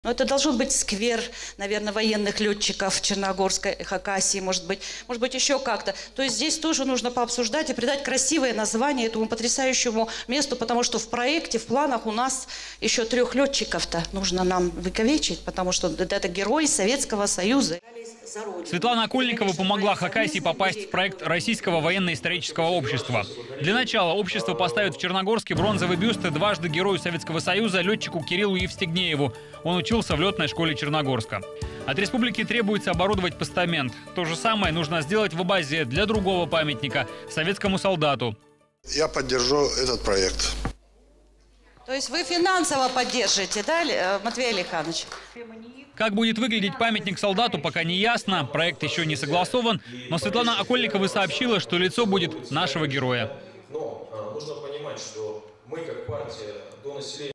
Два новых памятника появятся в Абазе и Черногорске
Российское военно-историческое общество при помощи Единой России поставит памятники в Хакасии